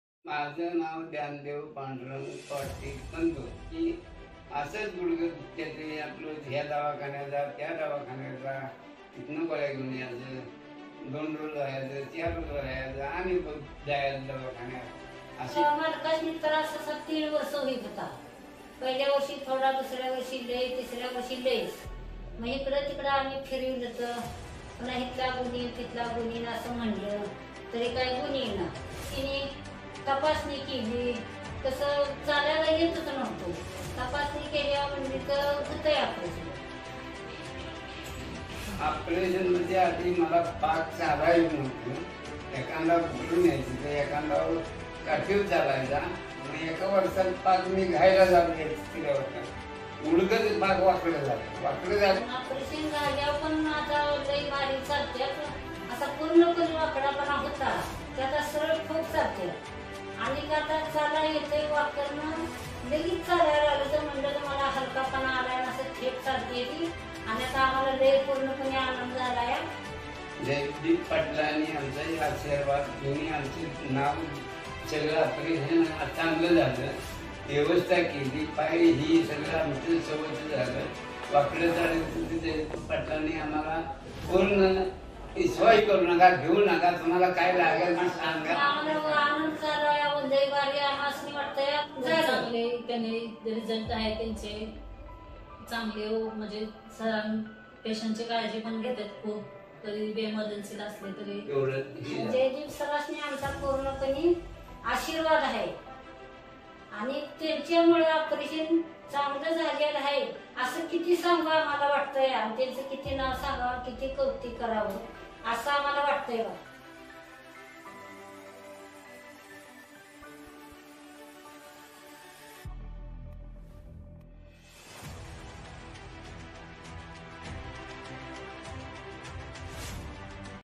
Total knee replacement (गुडघा सांधारोपण)|best results|Dr.Jaydeep Patil
This patient was having severe varus deformity with defect
         we have operated on him with both sides  total knee replacement with extension rod , The patient is walking normally with no previous deformity. The patient is so happy After getting his normal walk.